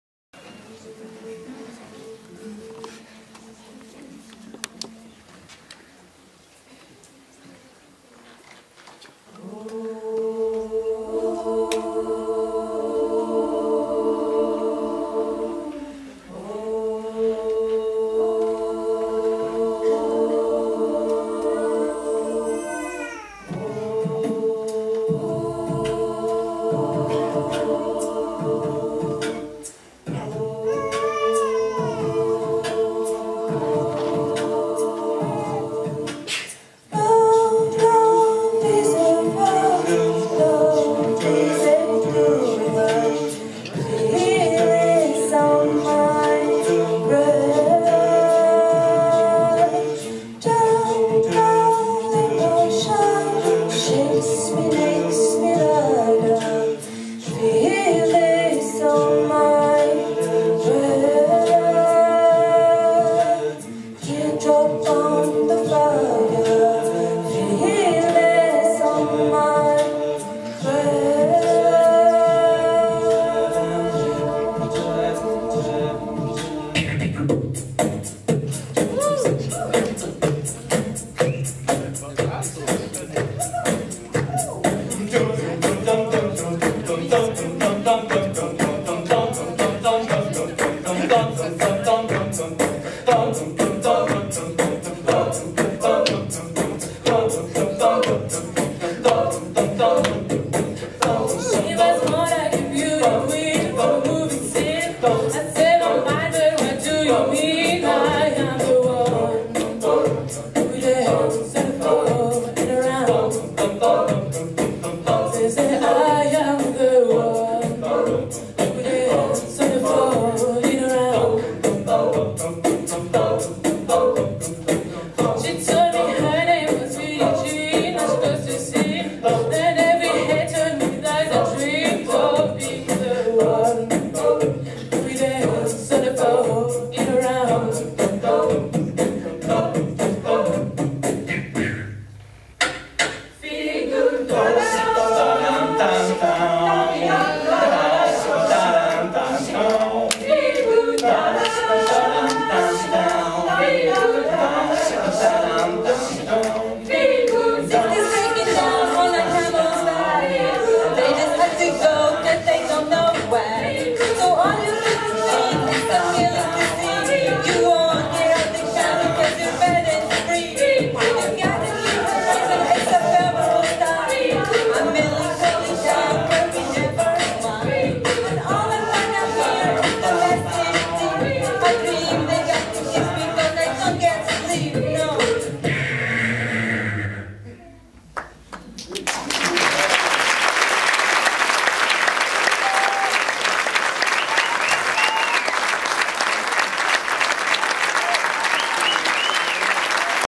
Medley UniVoice Band - Cover Massive Attack / M. Jackson / Gorillaz
Medley de covers, présenté lors du concert au Petit Kursaal de Besançon, le 05 juillet 2016